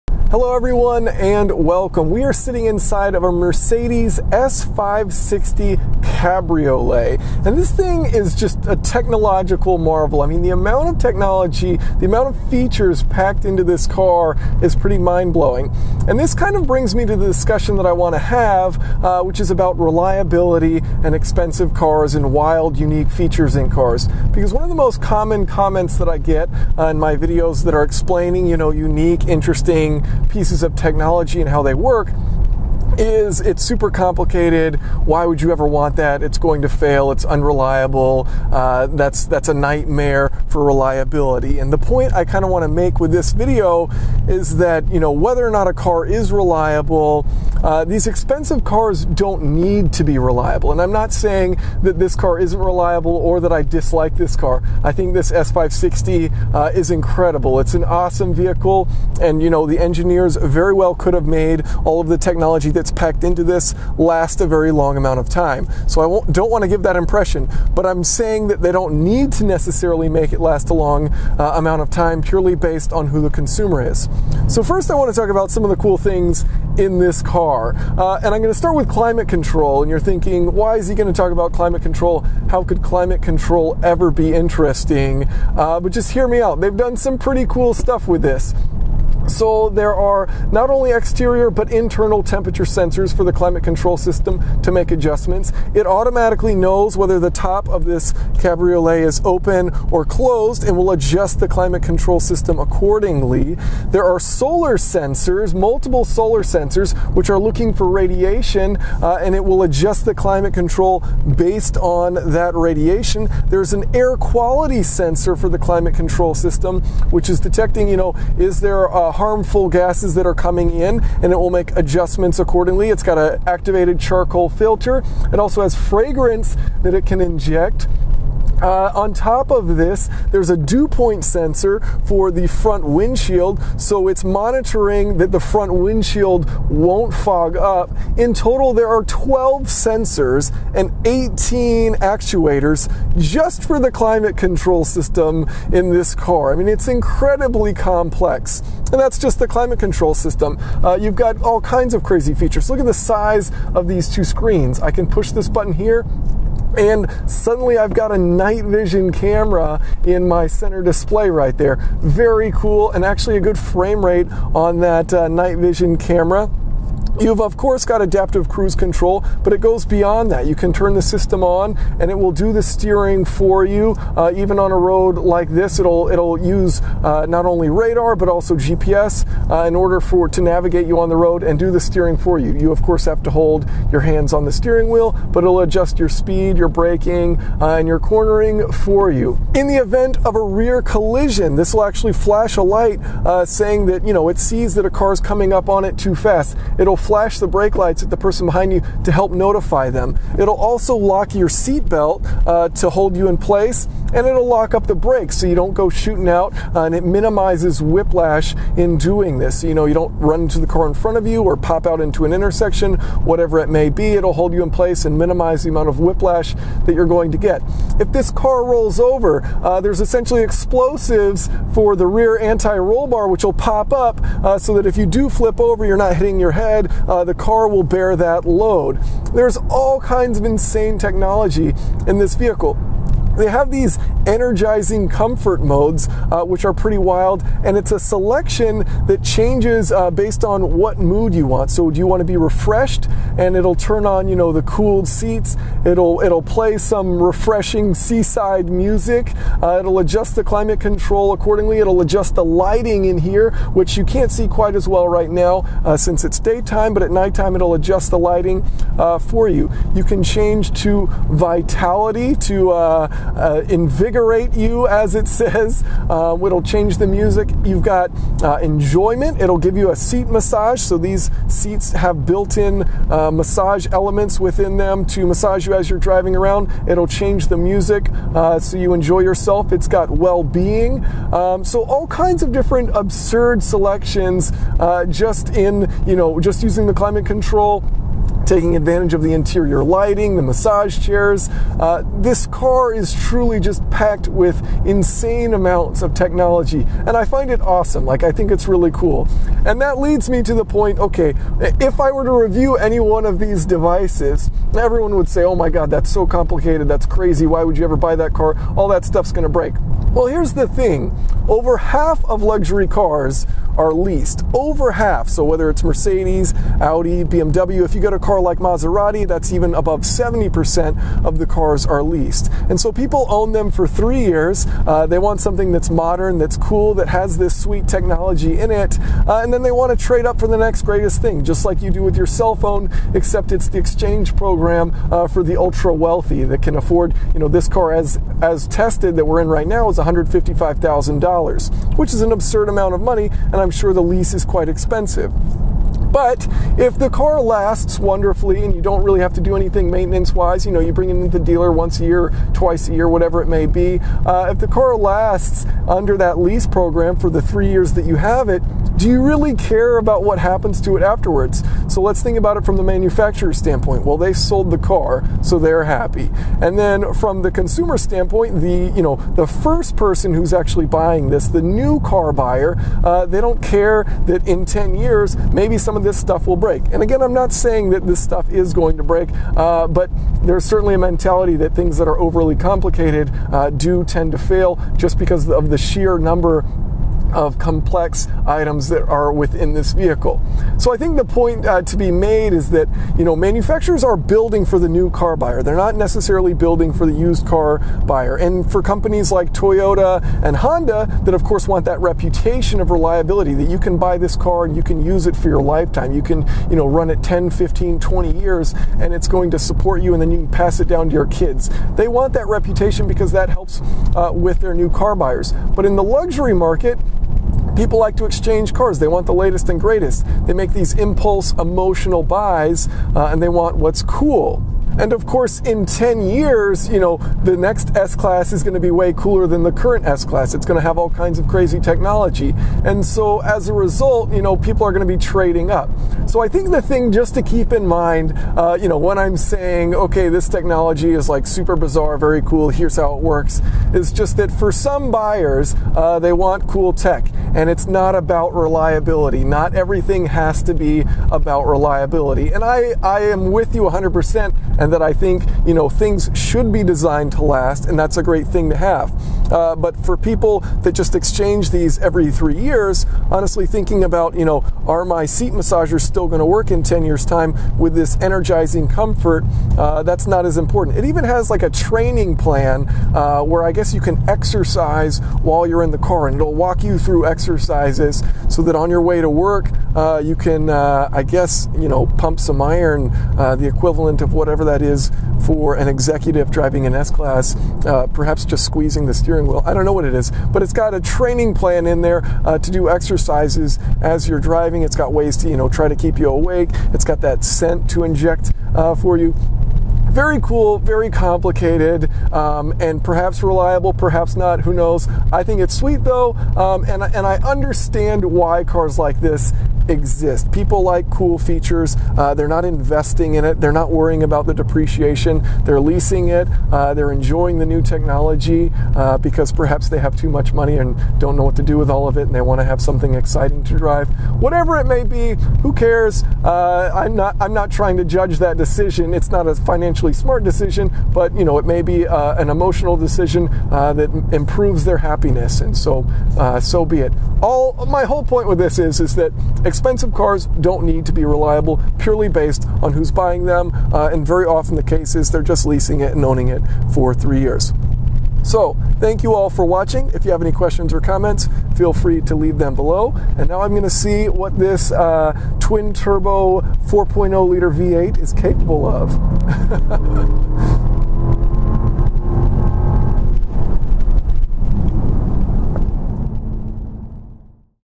Why Expensive Cars Aren't Always Reliable
Why Luxury Cars Don't Need To Be Reliable
It's Too Complicated, It Won't Last, It's A Mechanical Nightmare?

How do cars work? It's a simple question, though the answer is quite complicated. And it varies tremendously based on what car we're talking about. A $20,000 compact is far more simple than the $155,000 Mercedes S560 Cabriolet driven in this video. With that extra cost comes luxuriousness, the latest technology, and all kinds of features that most drivers will never even know the car has.

Often, complexity is associated with unreliability, and though they may often correlate, that doesn't mean complexity causes unreliability. In the case of luxury cars, it's important to consider who the buyer is. In this video, we'll discuss why it may be a win-win for both manufacturers and consumers to have expensive cars that aren't necessarily reliable.